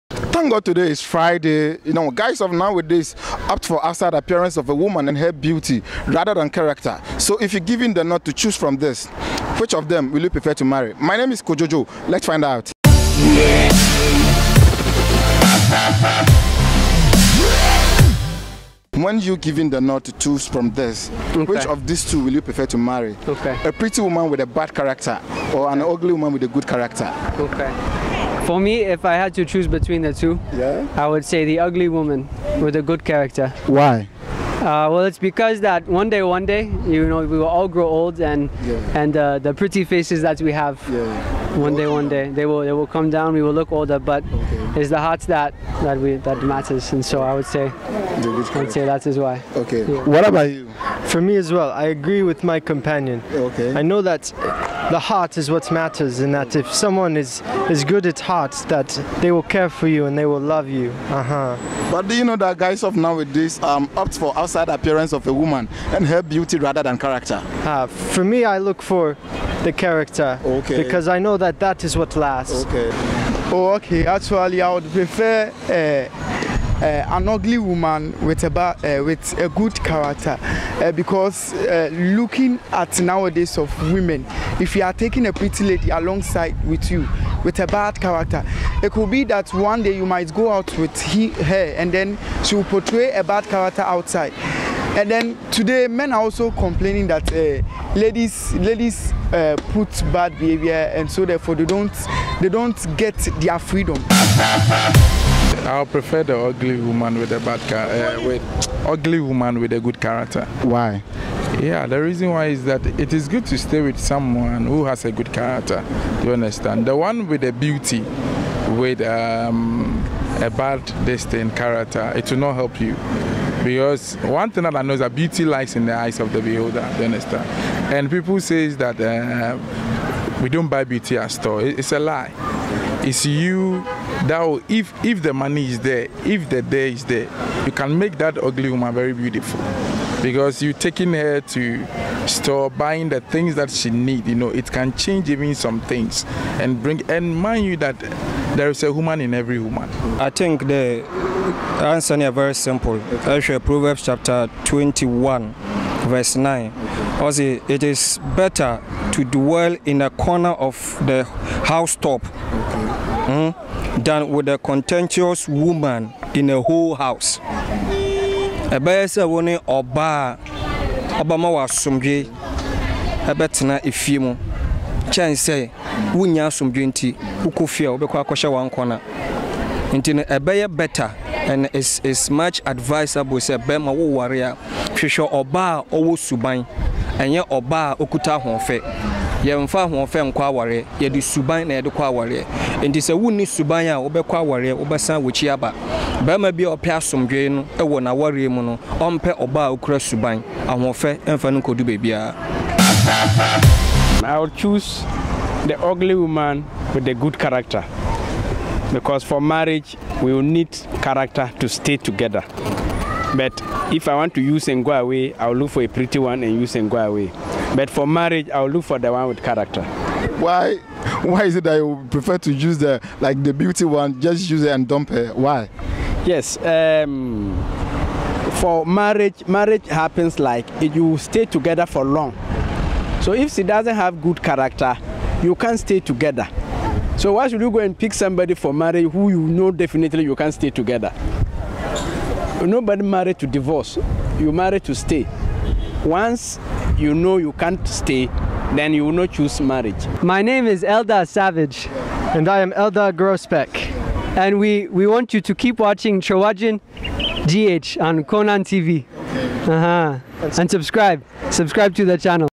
Which of this two will you prefer to marry?
If u are given the nod, which of thus two will u prefer to marry? A pretty women with a bad character or an ugly women with a good  character .